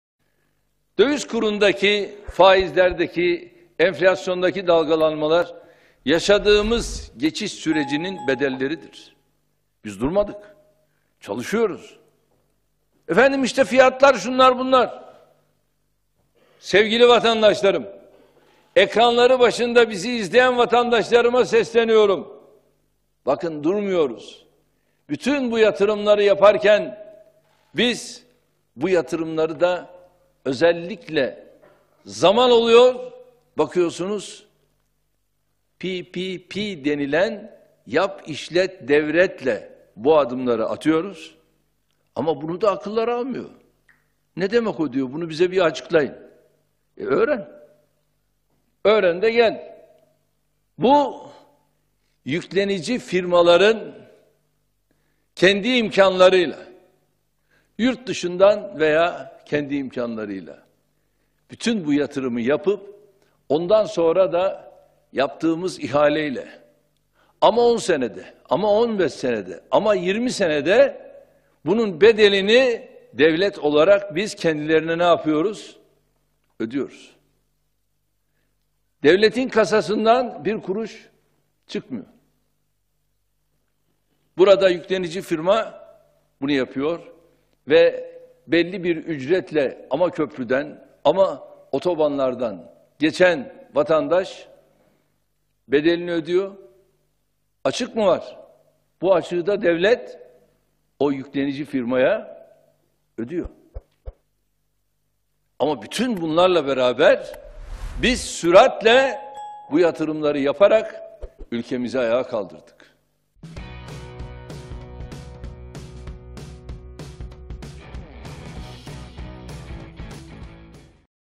Erdoğan'dan döviz kuru, faiz ve enflasyon mesajı
Cumhurbaşkanı Recep Tayyip Erdoğan, döviz kuru, faiz ve enflasyon ile ilgili açıklamalarda bulundu.